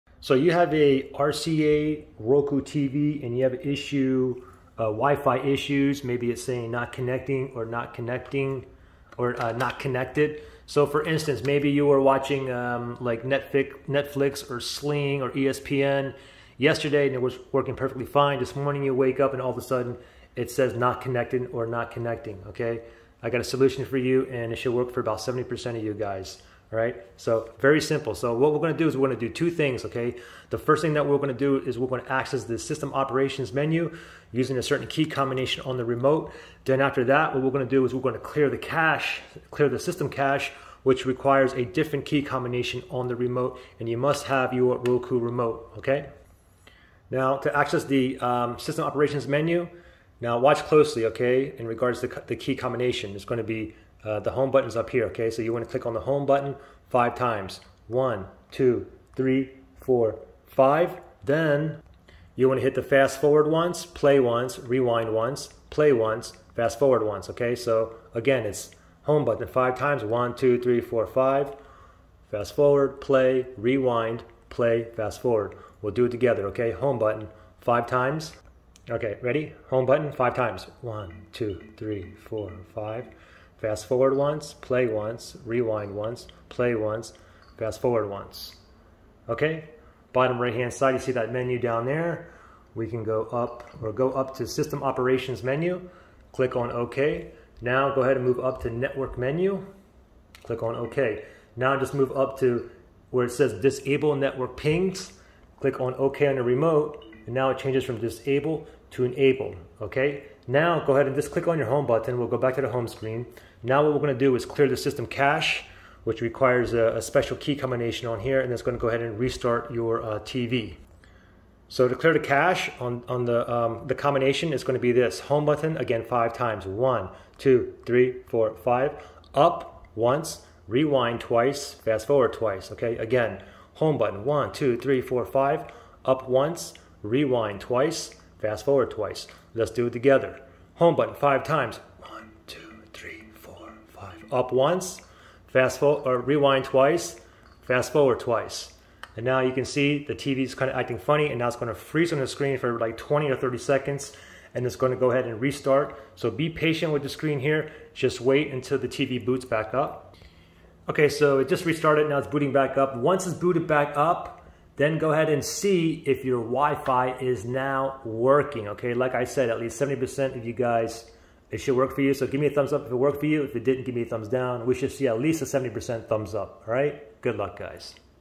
RCA Roku TV: WiFi Internet Network Not Connection (Not Connected) FIXED!
I show you how to fix an RCA Roku TV (smart TV) where its not connecting to the wifi internet network (No Connection or not connected). This should work for at least 70% of you so plz LIKE if it worked and DISLIKE if it didn't work so everyone can see. Hope this helps.